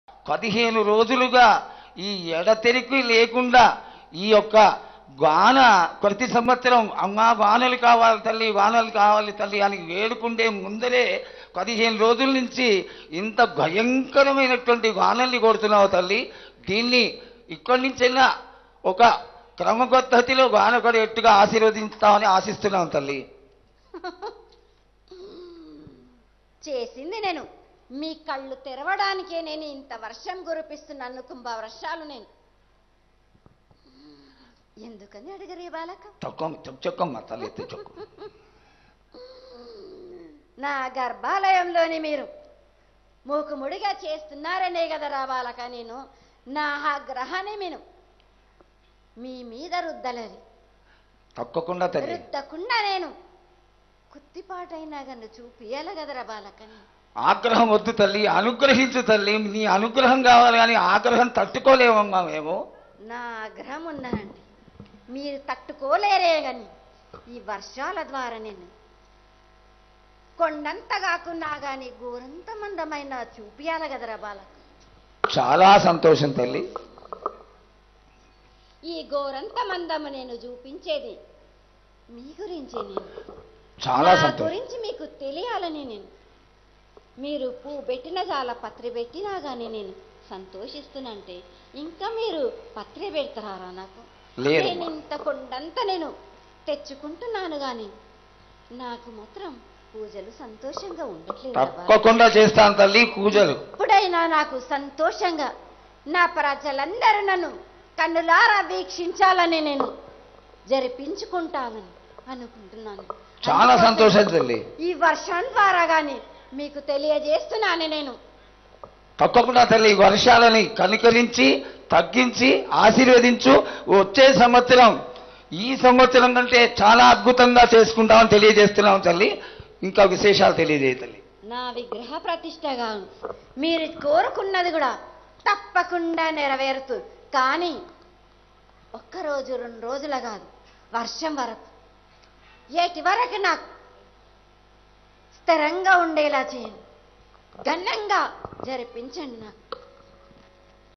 Swarnalatha predicts Bhavishyavani || నేను కావాలనే కుంభ వర్షాలు కురిపిస్తున్న - మాతంగి స్వర్ణలత
Matangi Swarnalatha : You are only performing Pooja for the benefit of your own well-being; you are not performing it in accordance with tradition.